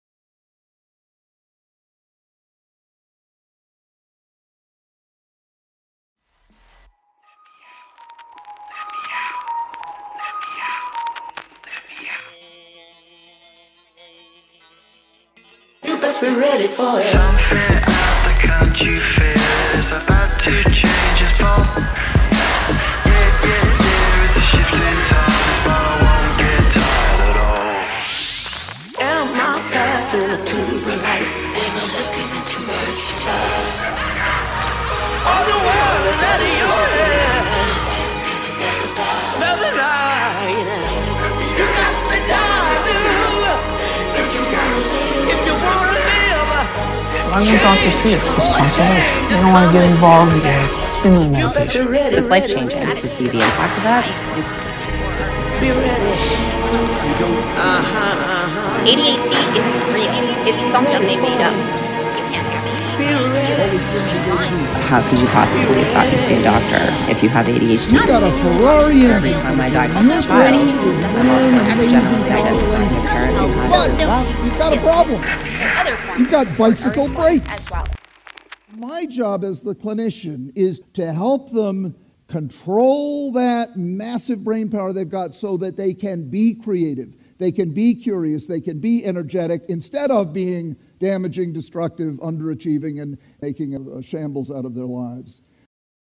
Animated Depiction of ADHD
Hi, I have ADHD and when I was asked to make an animation talking about a transdisciplinary topic I decided to make this.
I used a mixture of 2D animation, Traditional Animation rotoscoped over 3D Animation, Stop-Motion and Puppetry to make this.